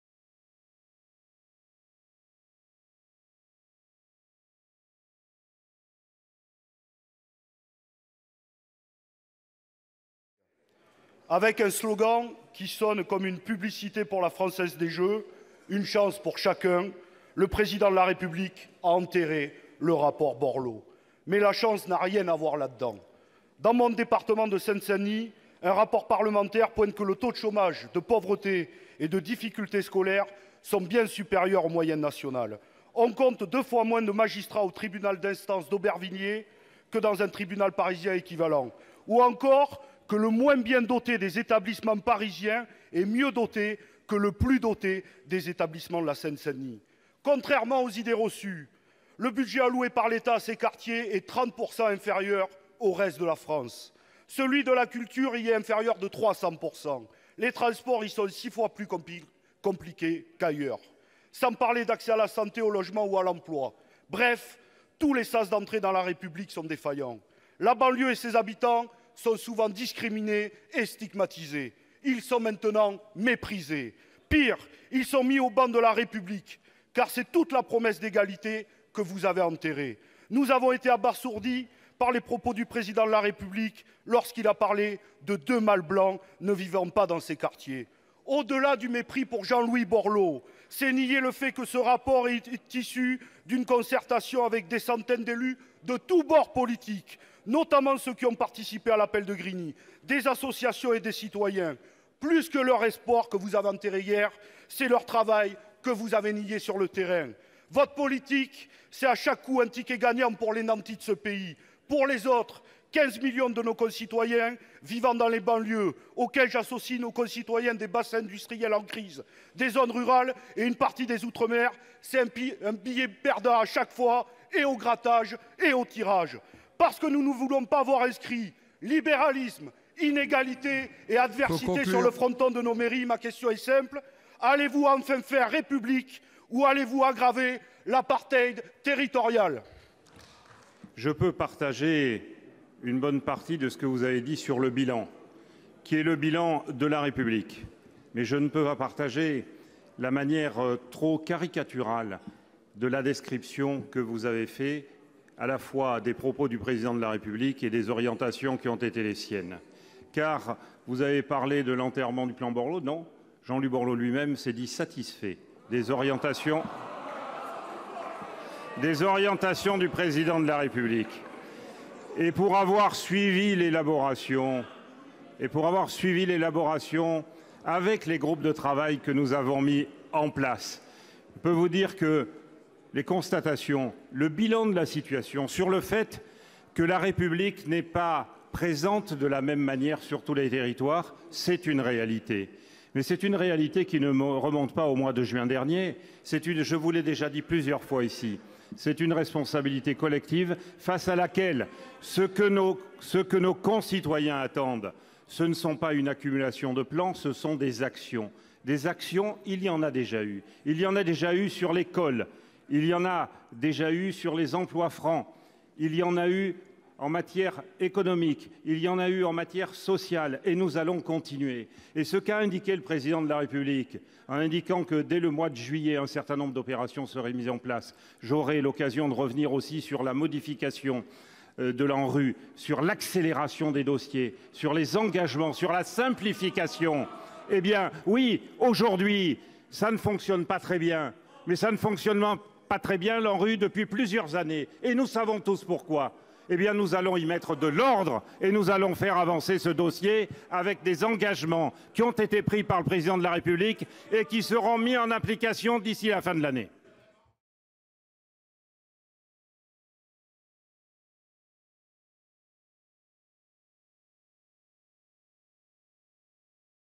La banlieue était déjà stigmatisée, elle est désormais méprisée
Question d'actualité de Fabien  Gay à propos du rejet du plan Borloo.